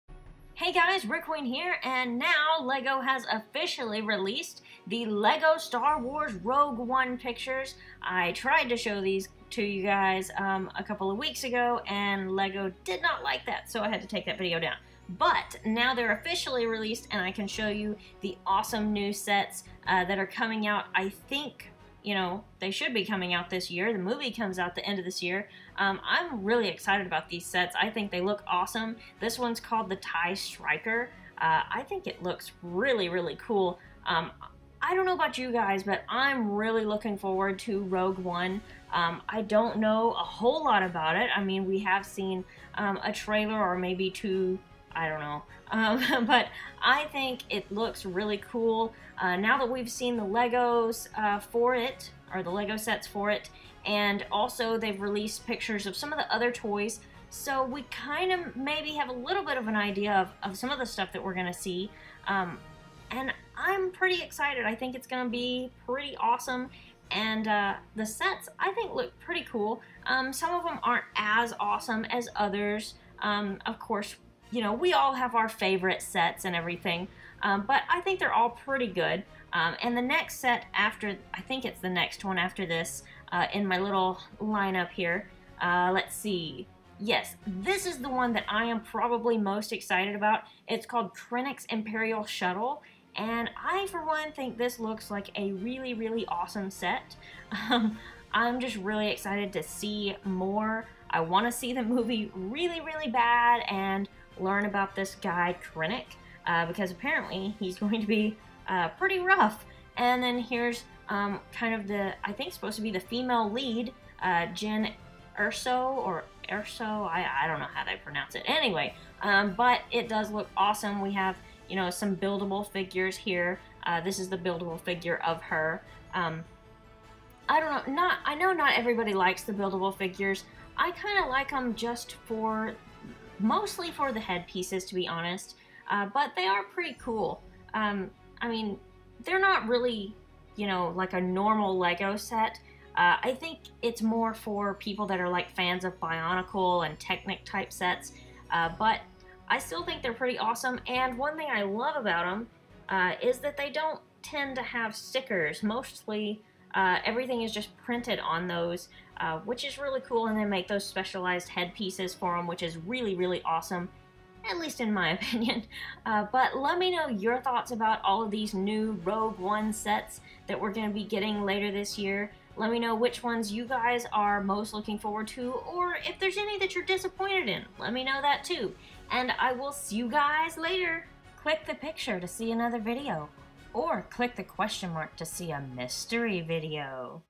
My Thoughts on LEGO Star Wars Rogue One Official Pictures - BrickQueen
LEGO Star Wars Rogue One set pictures have been officially released. Share your thoughts about these new sets in the comments!